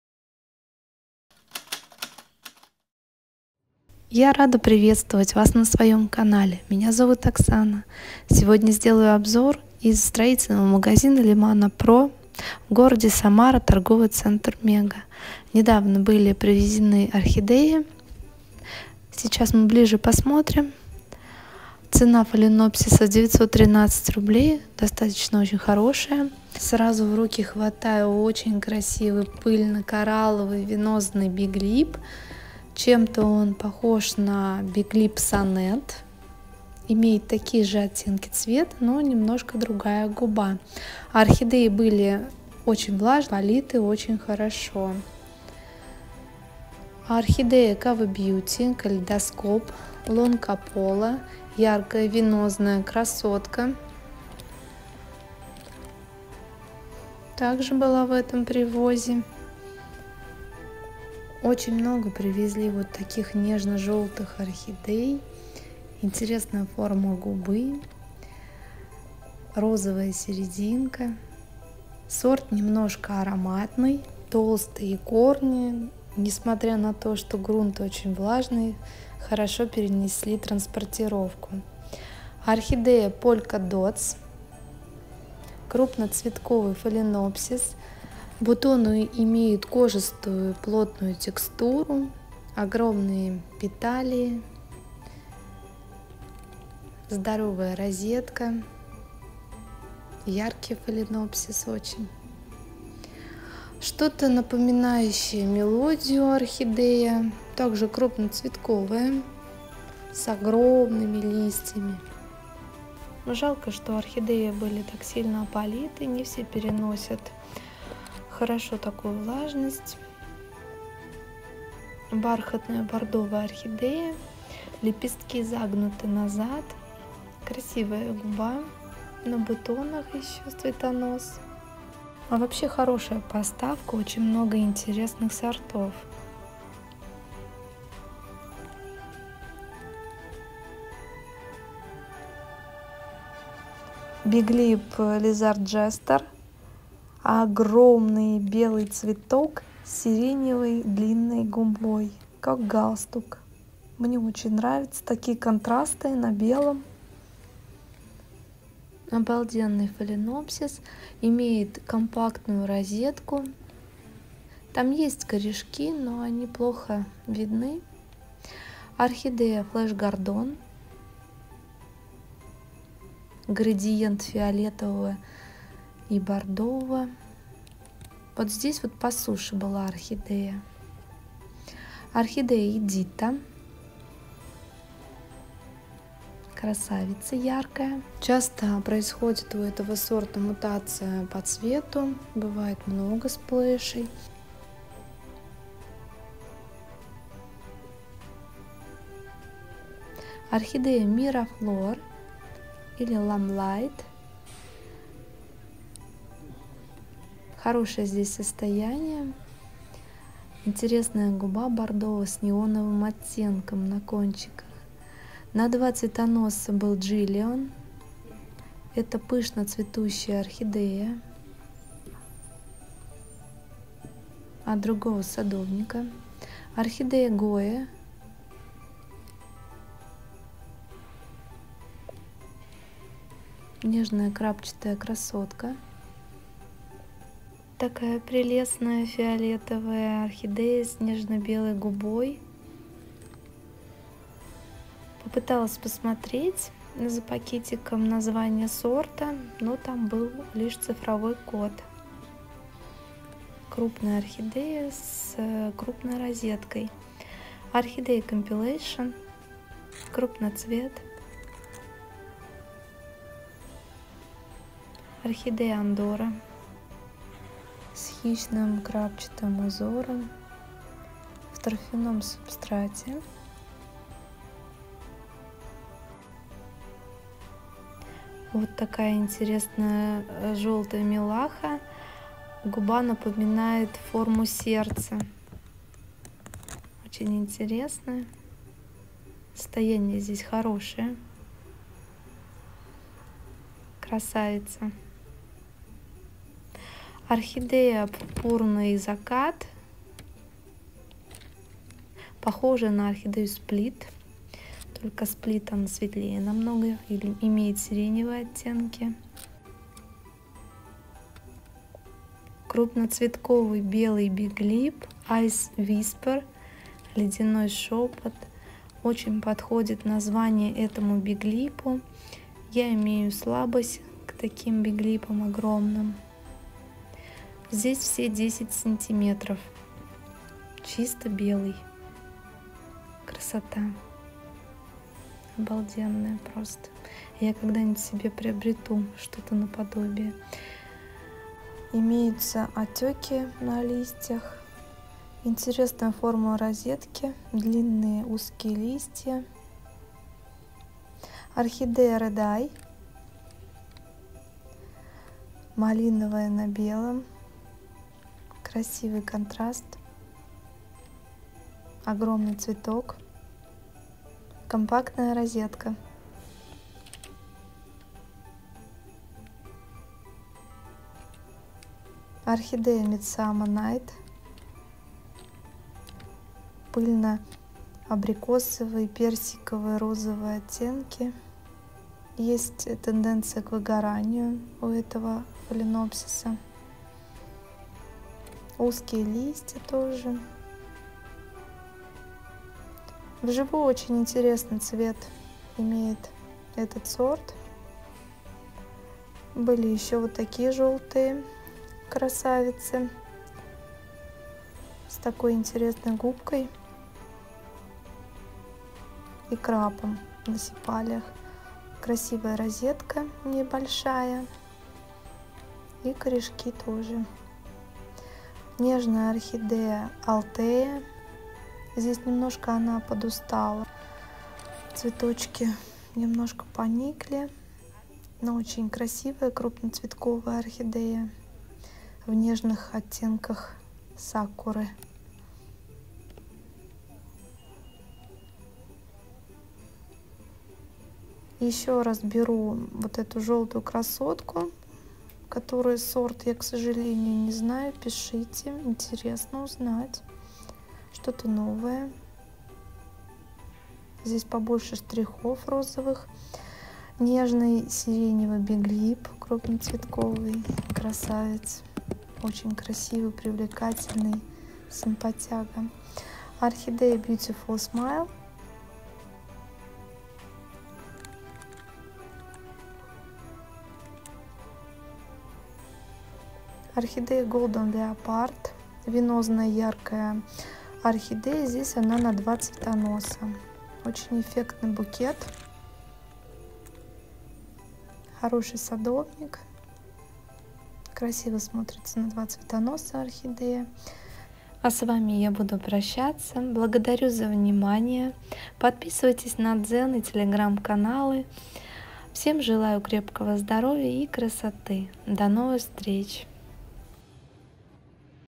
Красивые орхидеи биг липы в Лемана Про Самара ТЦ Мега - Ice Whisper, Midsummer Night, lizard Jester
Красивые орхидеи биг липы в Лемана Про Самара ТЦ Мега - Ice Whisper, Midsummer Night, lizard Jester, Edyta, Polka Dots, Flesh Gordon…Обзор от 12.11.24